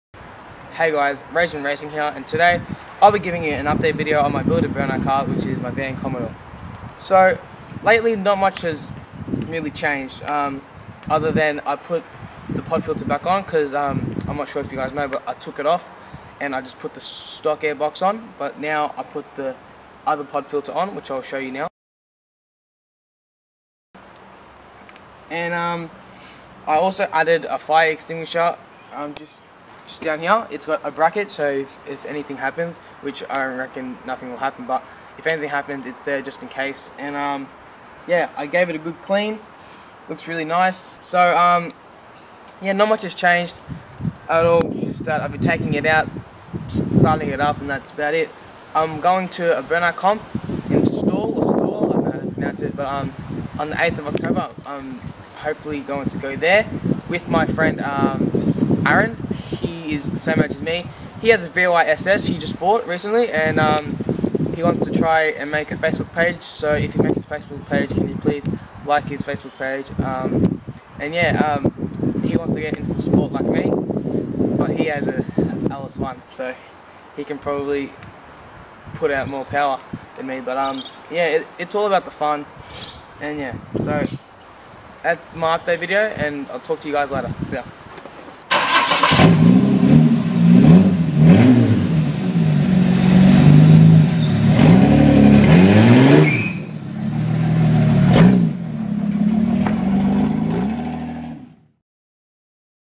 Build Burnout Car - LILSHT - Update Video
Hey Guys,
I do a quick update video on my Vn Commodore and explain if I'm going to burnout Comp and whats been added to my car.
KEEP WATCHING FOR MORE!